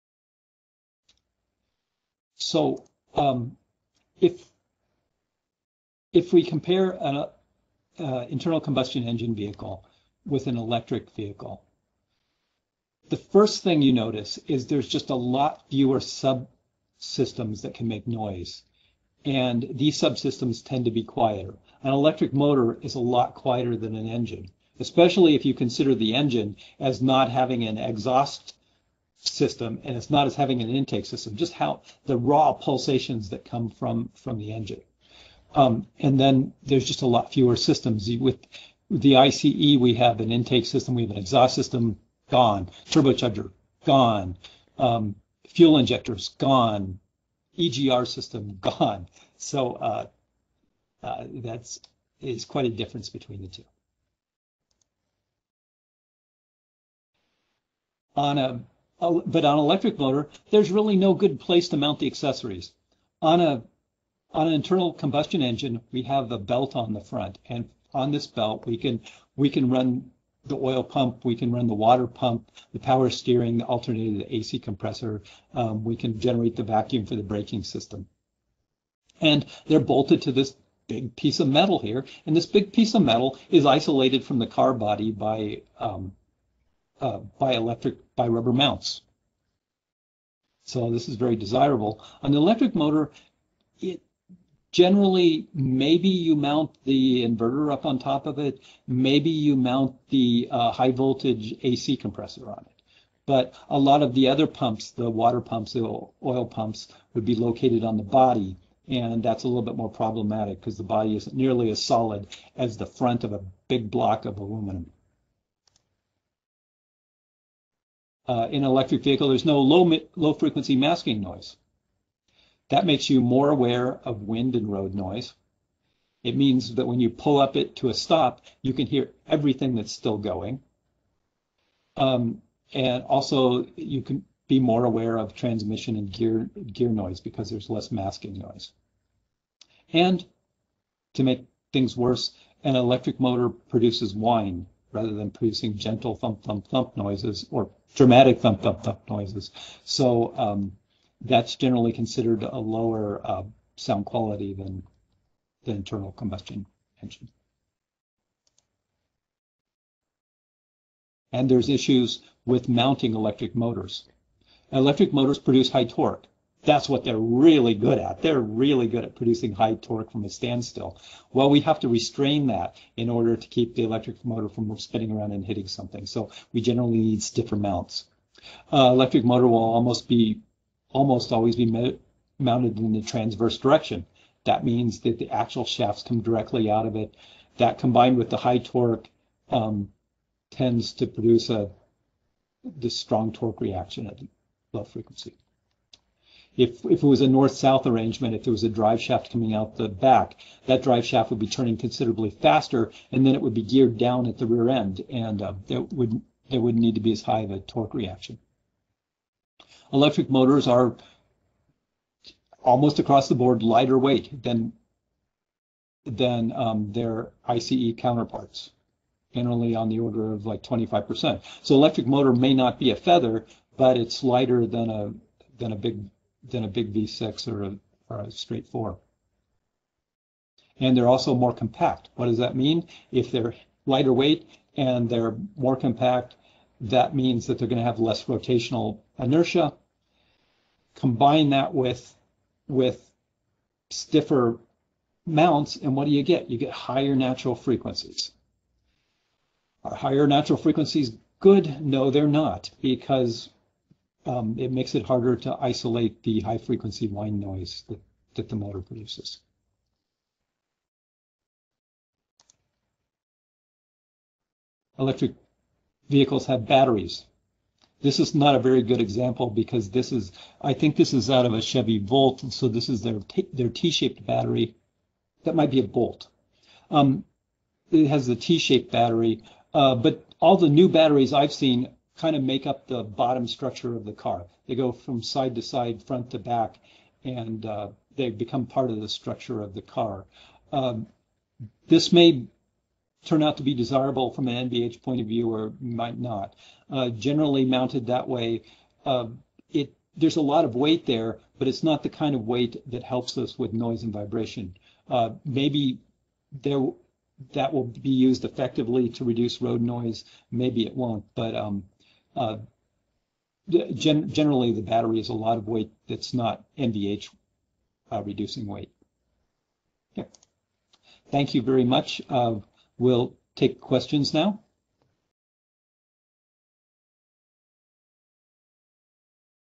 Comparing Noise and Vibration for ICE Vehicles and Electric Vehicles
Electric vehicles no longer have the mass or noise of engines to damp out sub system noises. This video will go over the differences between internal combustion engine noise and electric vehicle motor noise.